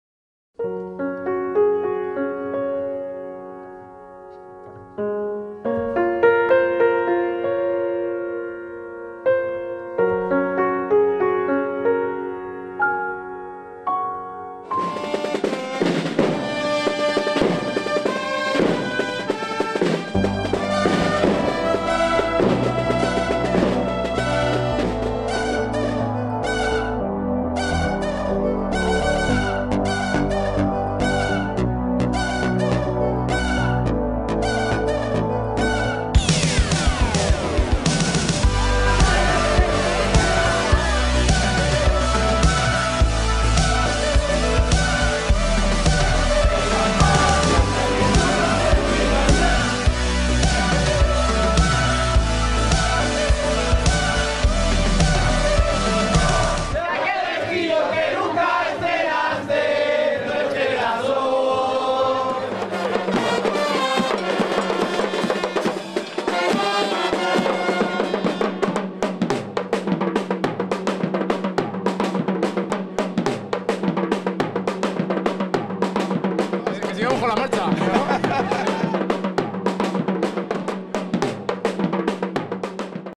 TRAILER CHULE Y BEA
CofrexProduce presenta el Trailer de la Boda de Chule y Bea celebrada en Cuenca.  Gente que lo da todo todo jajajaj